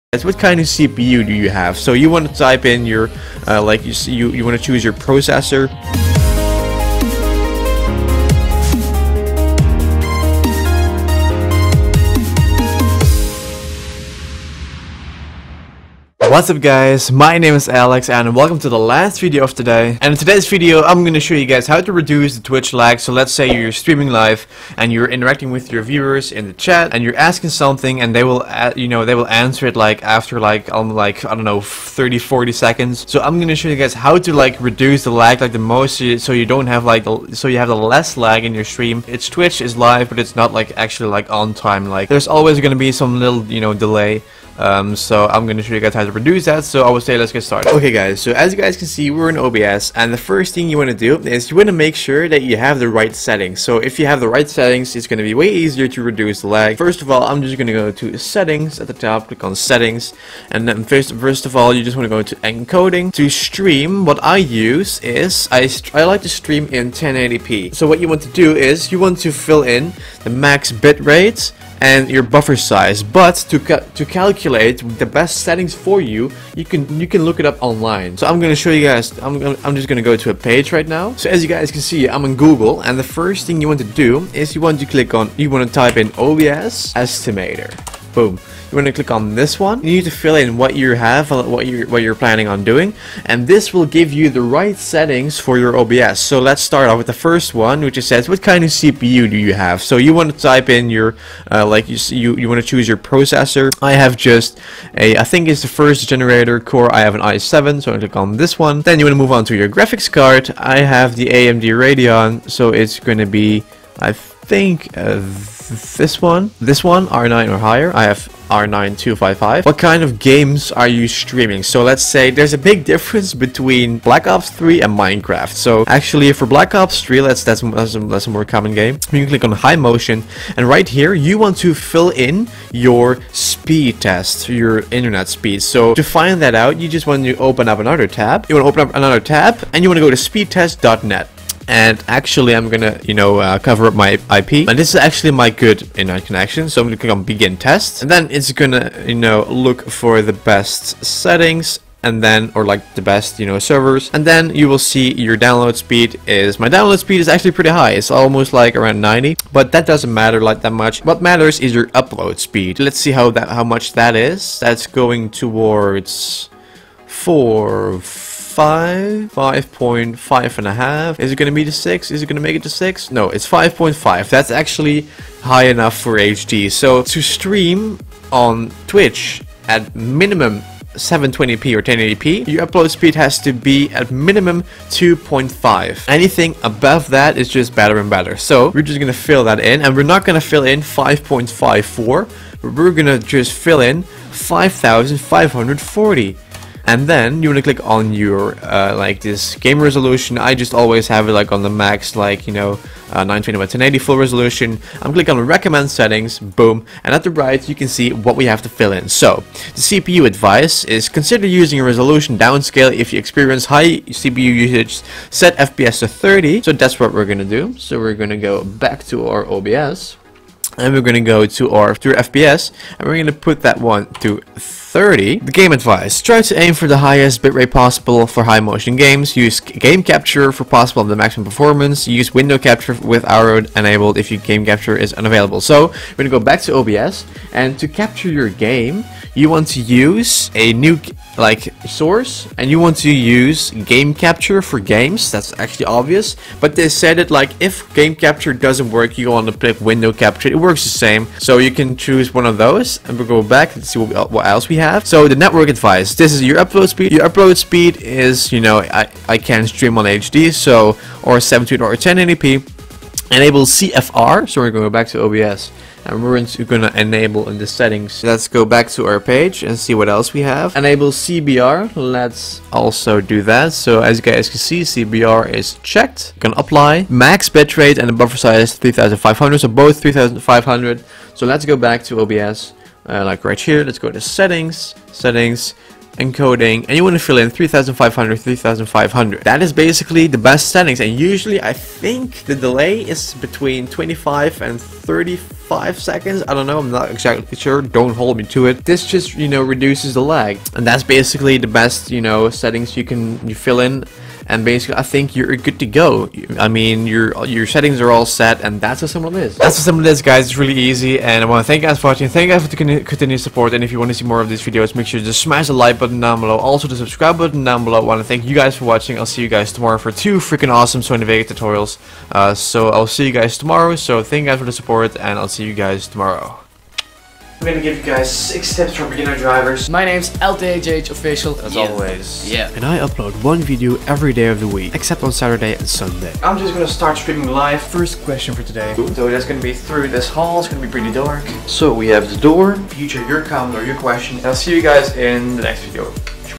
How To Reduce Twitch Streaming Delay In Open Broadcaster Software - Tutorial #28
- - In today's tutorial I'll be showing you how to reduce Twitch delay in Open Broadcaster Software. I receive a lot of comments and questions about how to do certain things. More tutorials are on its way!

~ JustAlexHalford